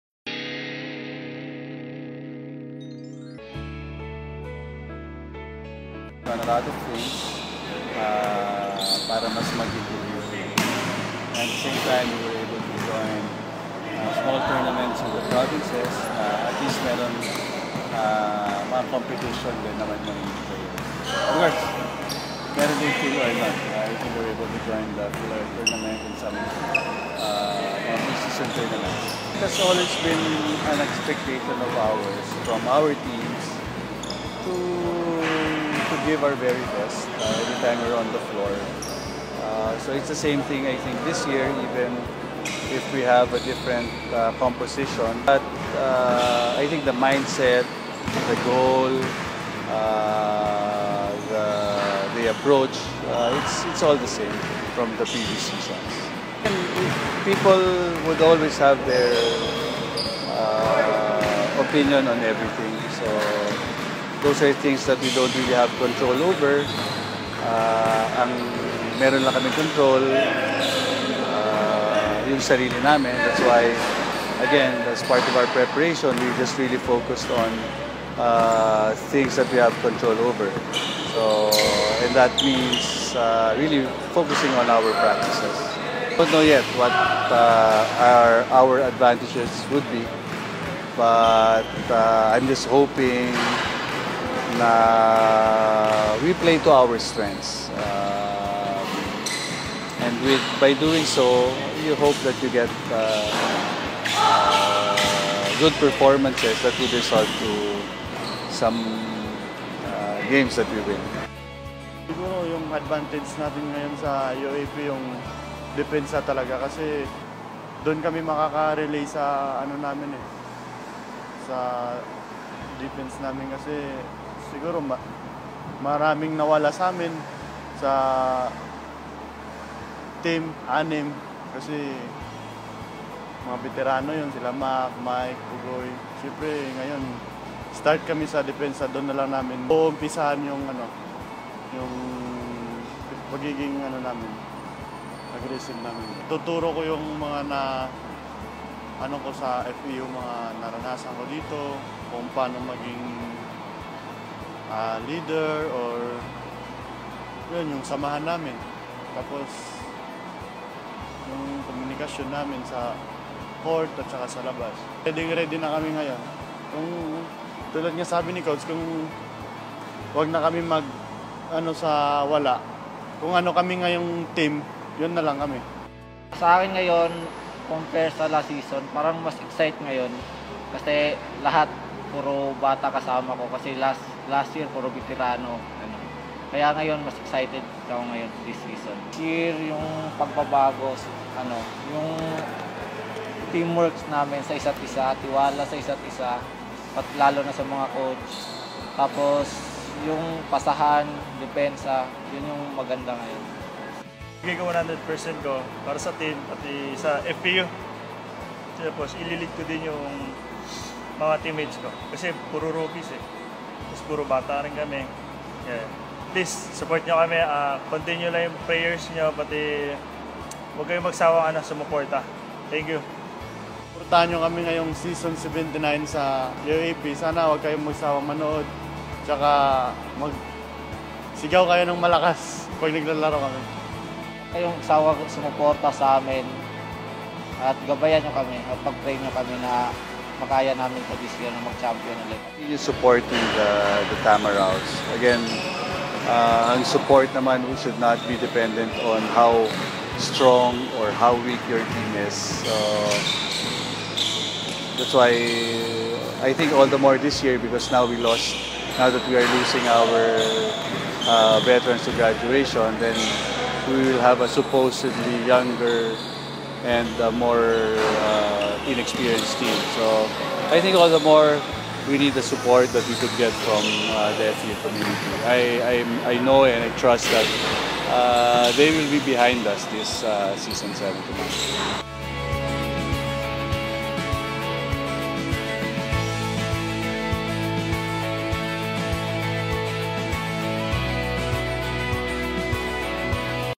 TAMSclusive: FEU Tamaraws - Season 79 Primer
FROM OUR ARCHIVES: A glimpse of Far Eastern University (FEU) Tamaraws' preparation for the University Athletic Association of the Philippines (UAAP) season 79 men's basketball tournament.

The video was originally uploaded through FEU Advocate's Facebook page, September 1, 2016.